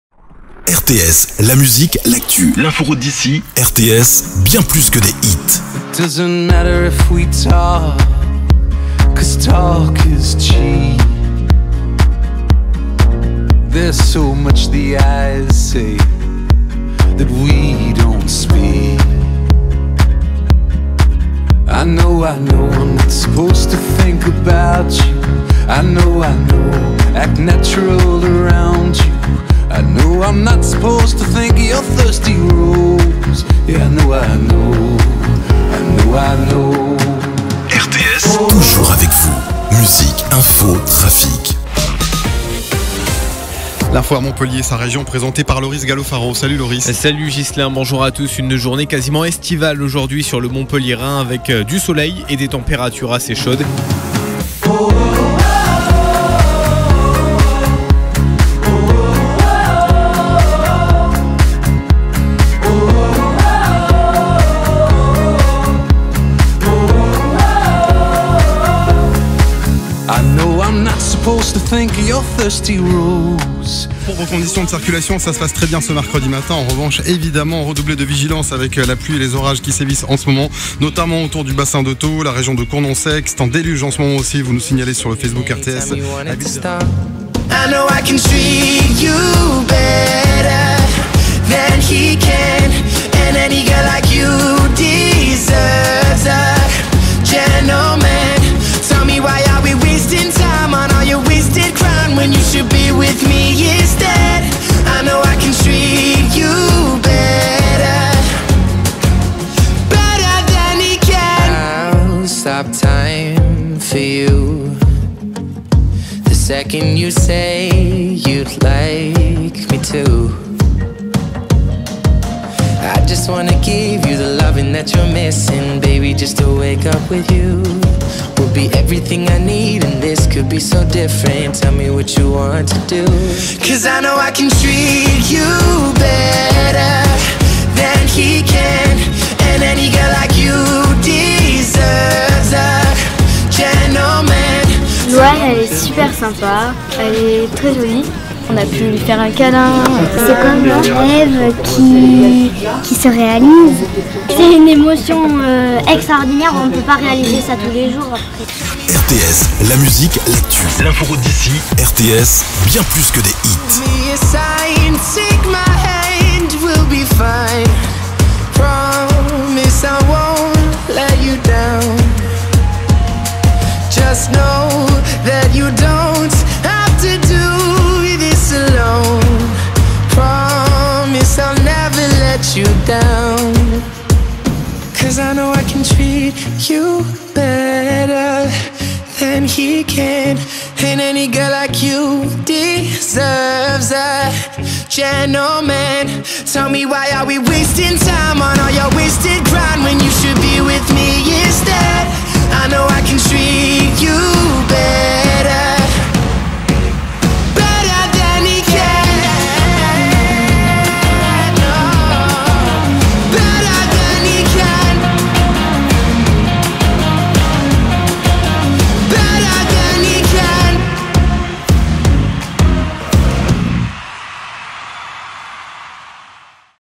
Découvrez RTS 1ère Radio Indépendante du Languedoc-Roussillon
RTS a 35 ans,  c'est la 1ère radio indépendante du Languedoc-Roussillon avec plus de 235 000 auditeurs hebdomadaire.

En visionnant cette vidéo vous allez découvrir en quelques minute ce qu'est RTS.

Et écoutez RTS partout en téléchargeant l'appli RTS sur iOs et Android